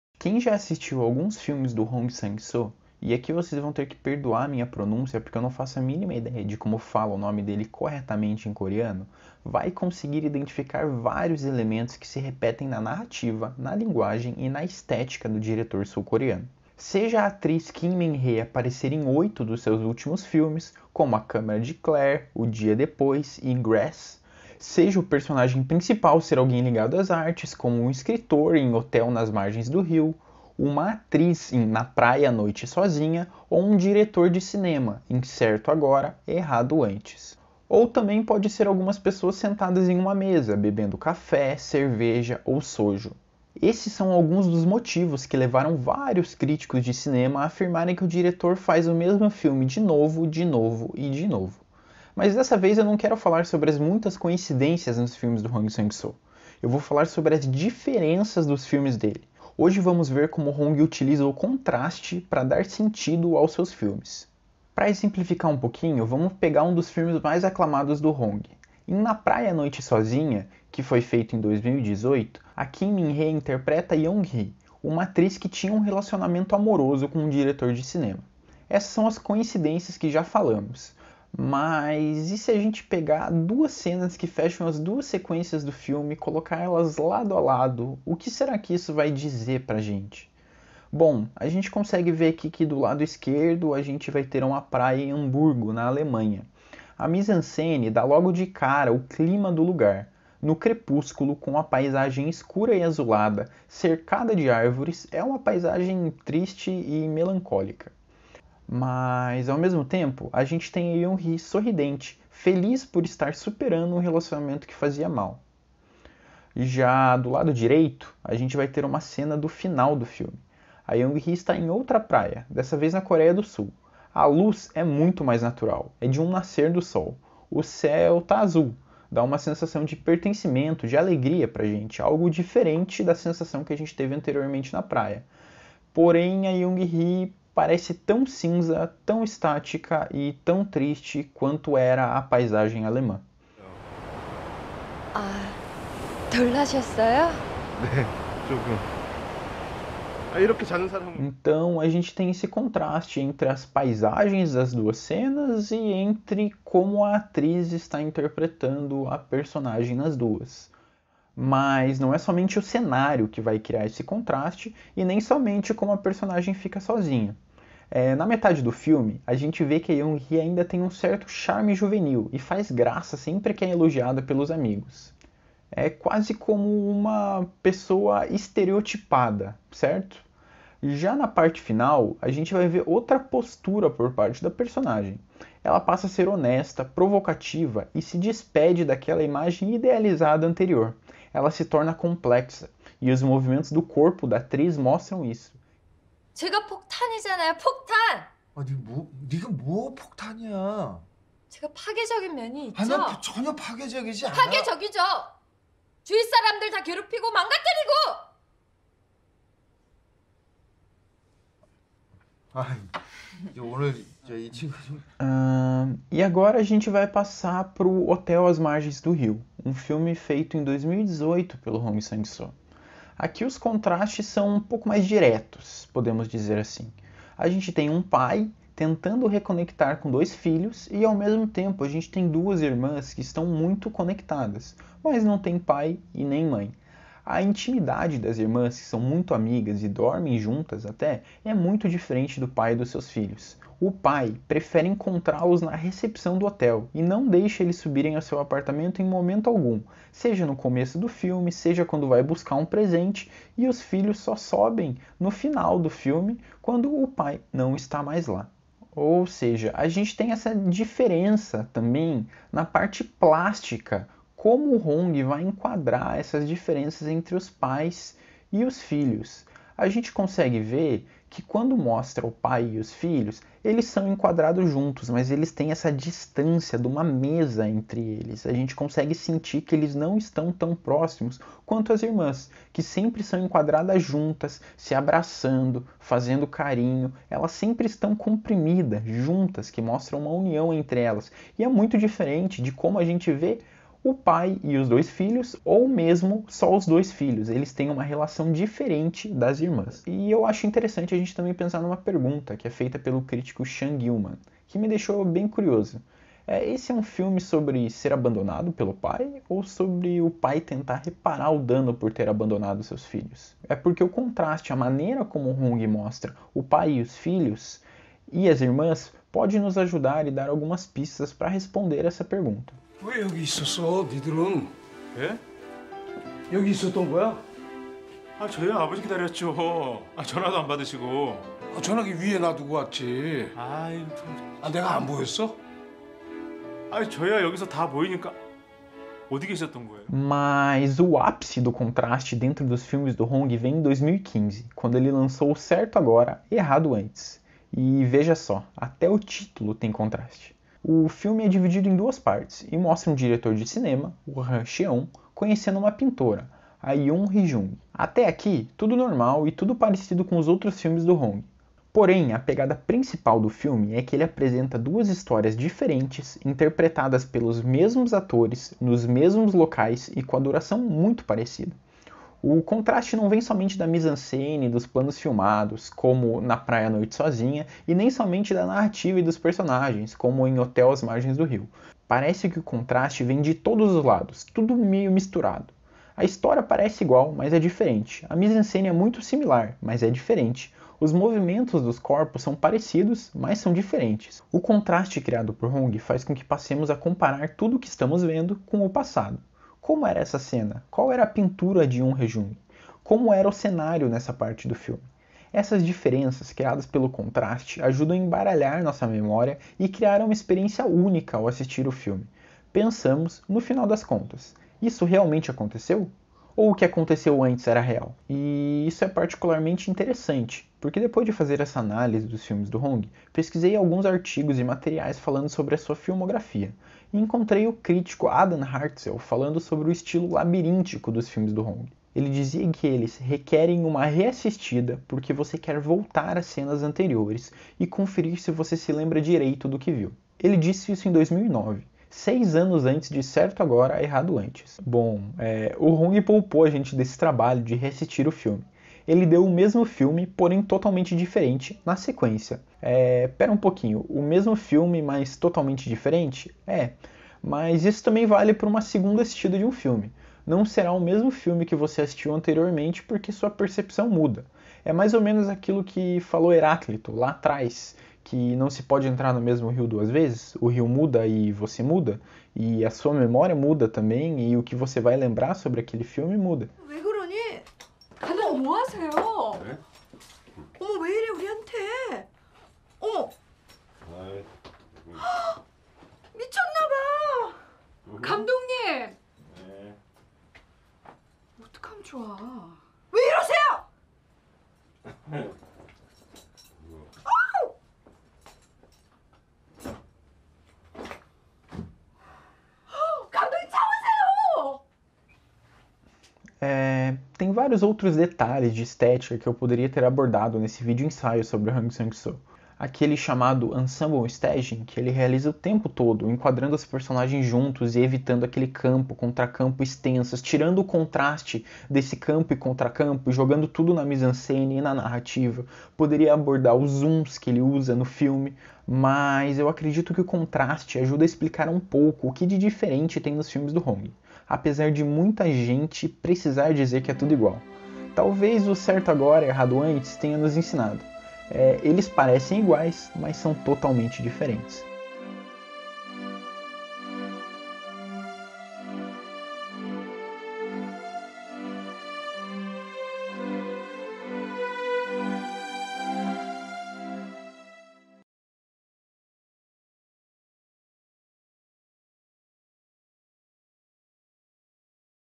o CONTRASTE em HONG SANG SOO - vídeo ensaio
quem já assistiu alguns filmes do hong sang soo consegue identificar vários elementos que se repetem na narrativa, na linguagem e na estética do diretor sul coreano. seja a atriz kim minhee aparecer em oito dos seus últimos filmes, seja o personagem principal ser alguém ligado às artes como um escritor, uma atriz ou um diretor de cinema, ou seja as pessoas sentadas em uma mesa bebendo café, cerveja ou soju.

esses são alguns dos motivos que levaram vários críticos de cinema a afirmarem que o diretor faz o mesmo filme de novo, de novo e de novo. mas dessa vez eu não quero falar sobre as muitas coincidências dos filmes do hong sang soo. vou falar sobre as diferenças dentro dos filmes dele. hoje vamos ver como hong utiliza o contraste para dar sentido aos seus filmes.

vídeo ensaio produzido para a disciplina de estética cinematográfica do curso de cinema e audiovisual da unespar (universidade estadual do paraná).